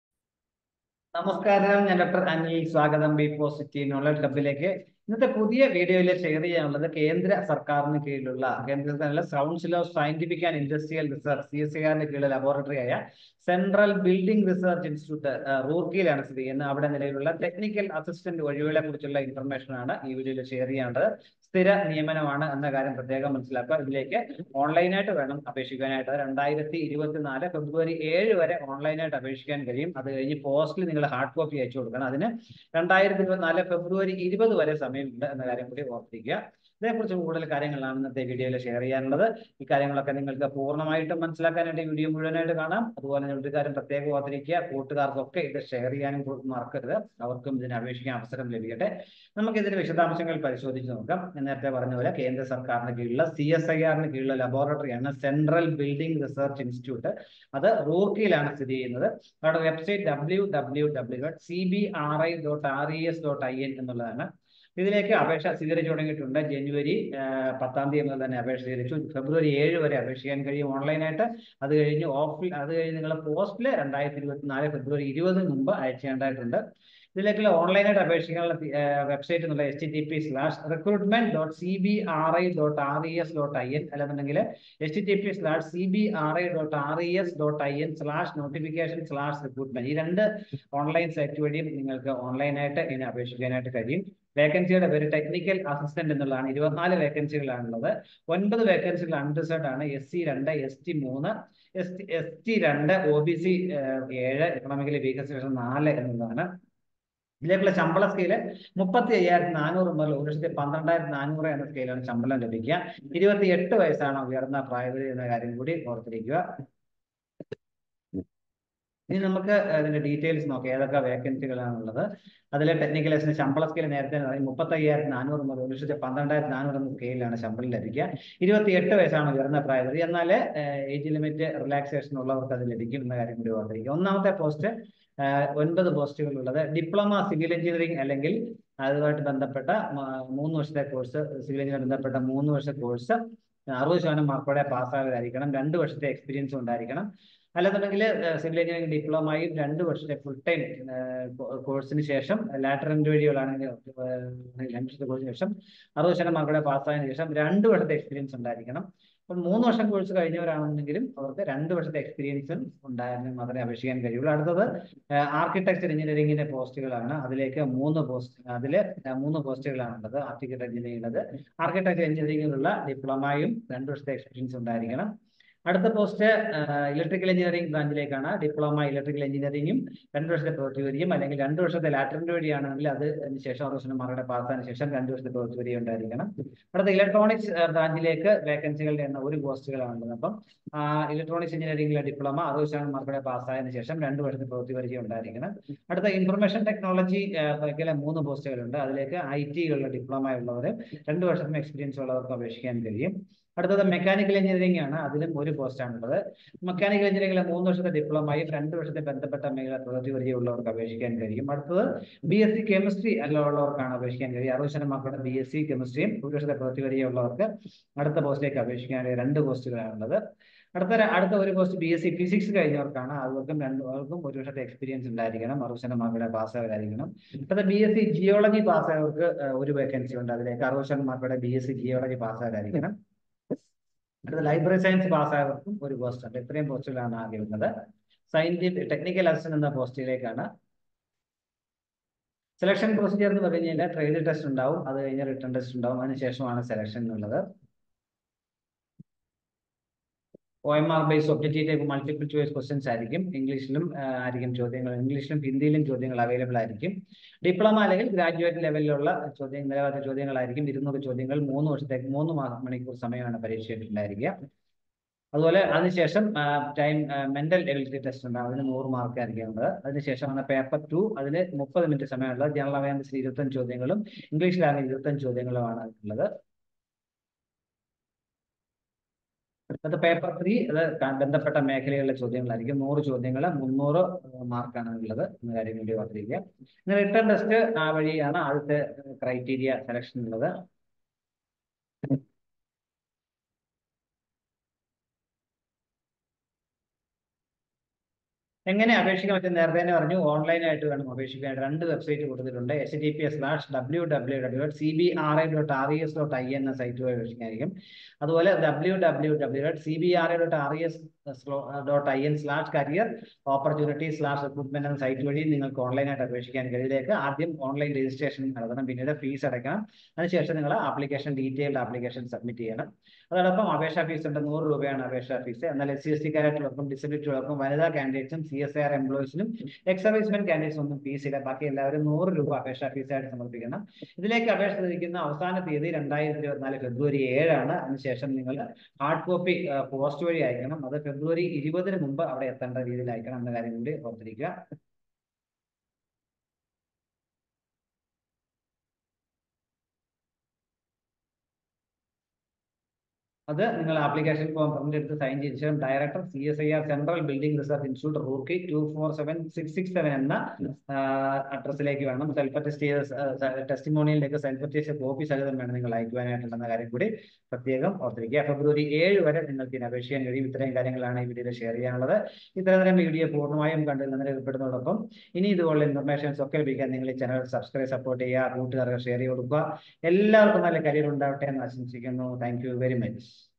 Central Building Research Institute-ൽ Technical Assistant
CBRI Recruitment 2024 – Apply Online For Latest 24 Technical Assistant Vacancies